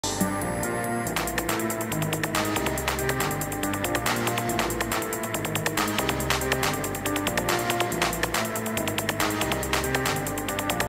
Sir Hijsalot - Animation of Life
Animaties van mij en Gijs op een stukje Aphex muziek